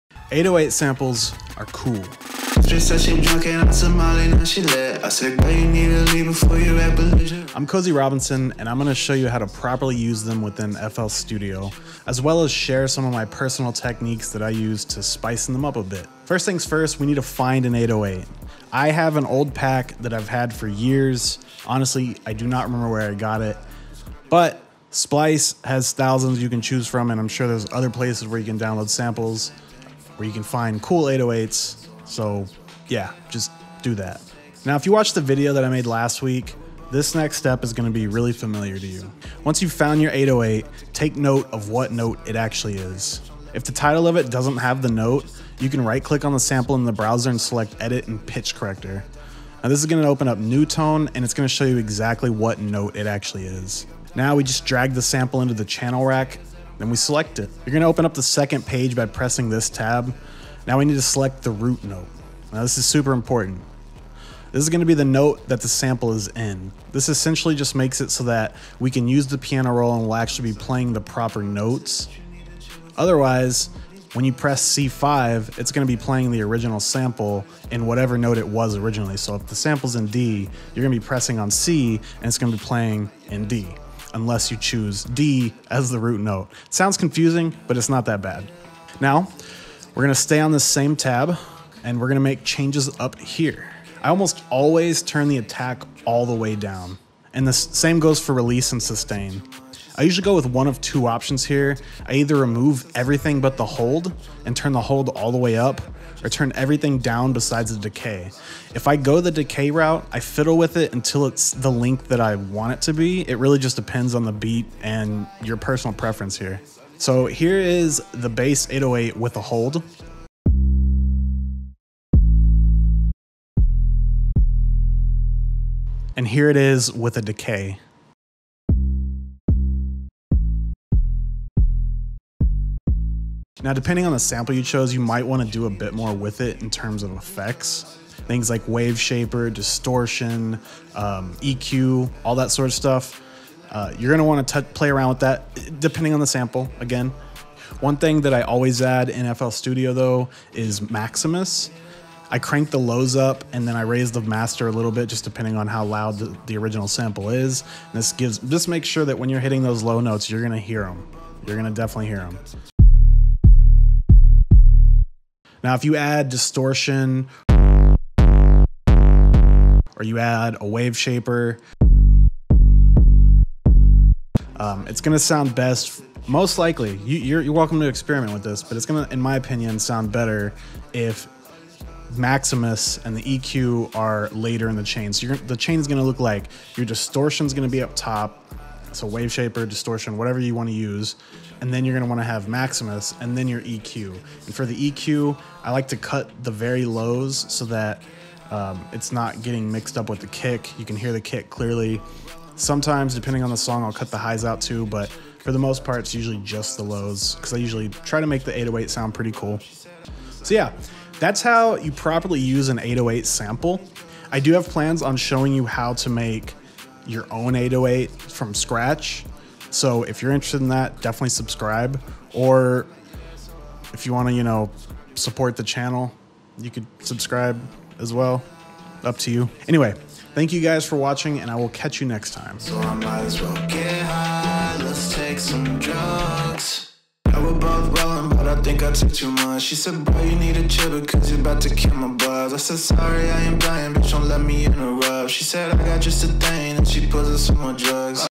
How to properly use 808 samples in FL Studio
Learn how to properly use 808 samples in FL Studio with ease! Super simple.

Previously known as yec0dy / Thrilla.